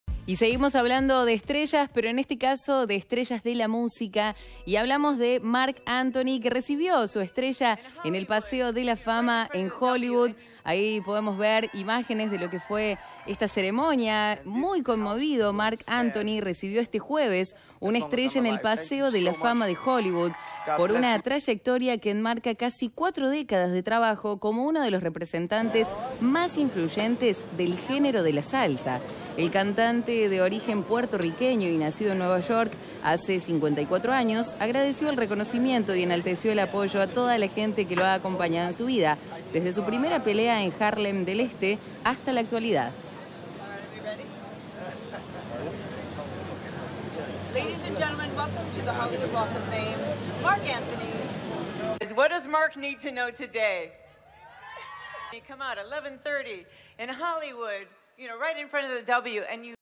Marc Anthony ya tiene su estrella en el Paseo de la Fama de Hollywood
👉 El cantante de origen puertorriqueño y nacido en Nueva York, recibió su estrella en el paseo de la fama por una trayectoria de casi cuatro décadas como representante del género de la salsa.